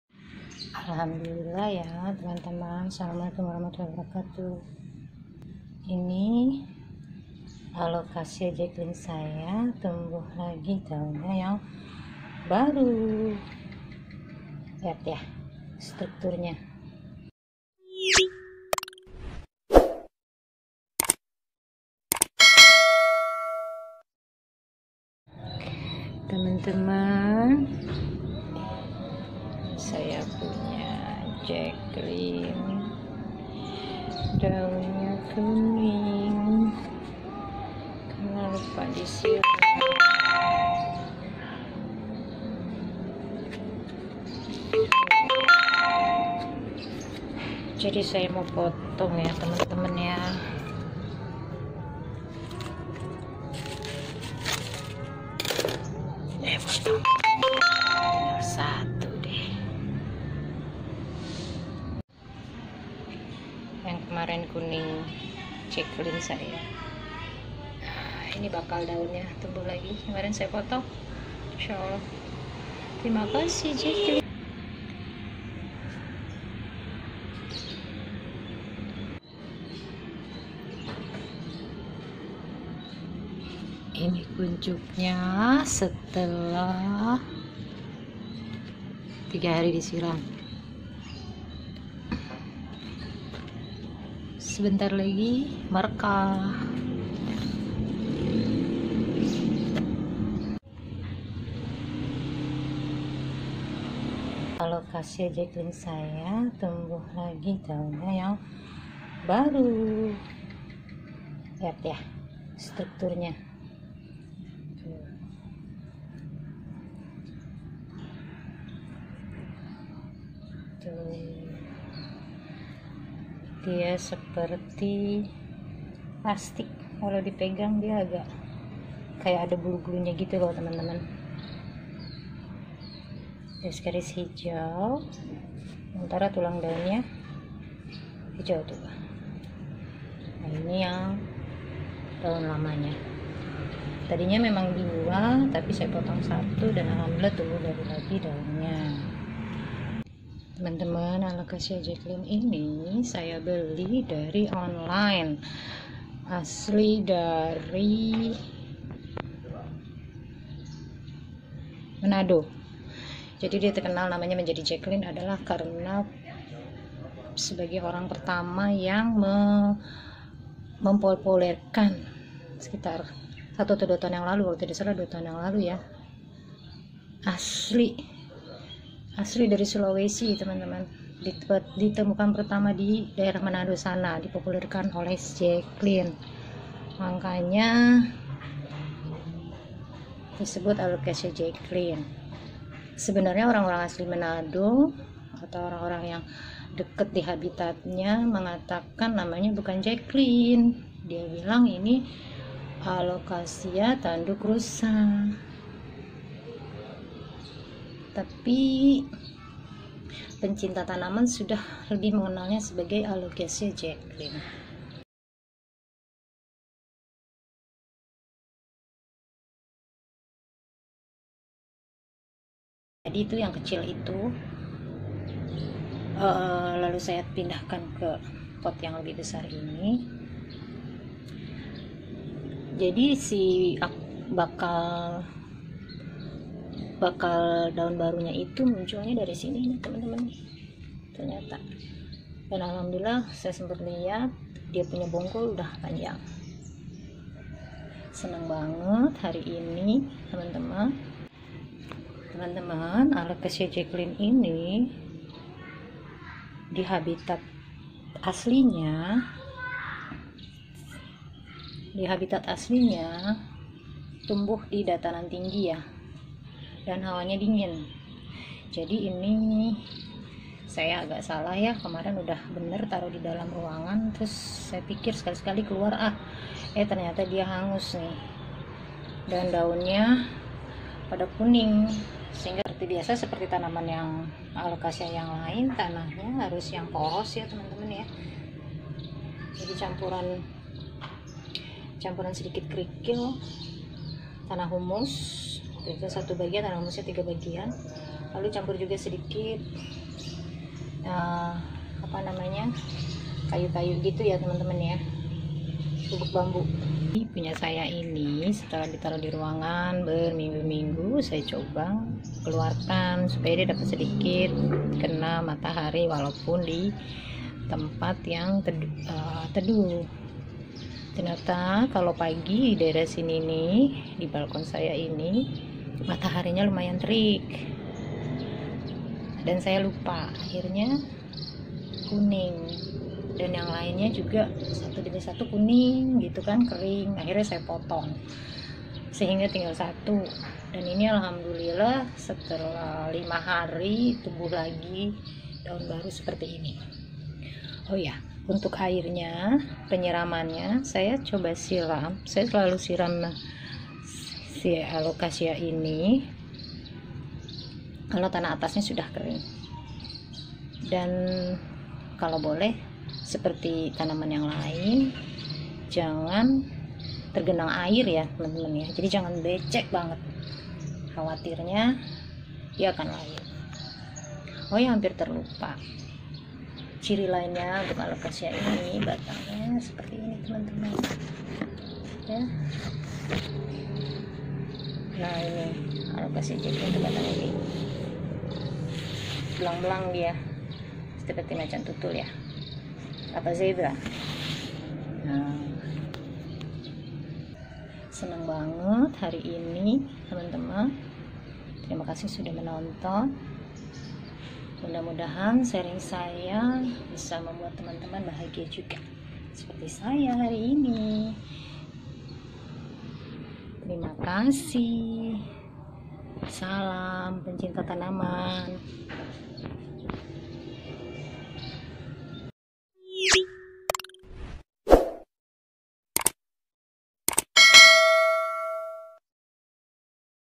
Alocasia Jacklyn menguning | Alocasia Jacklyn care
Bagaimana menanam tanaman hias. Memelihara tanaman hias. Propagasi dan memperbanyak tanaman hias
 Bahagianya melihat tanaman cantik. Hoya kaktus dan sukulen.

Alocasia Jacklyn was started famous at the first time about two years ago.  Ms.Jacklyn did it, and name it with her name.  She propagating it and spread the plant to others plants lover.
Alocasia jacklyn original habitat found in North Sulawesi - Manado.  The people there call him " alocasia deer horn".  He need following condition to care :
1. Soil , mix it with sand, 3 : 1
2. Watering when the top soil dry
3. Put him in door, or under the big plant ,

Salam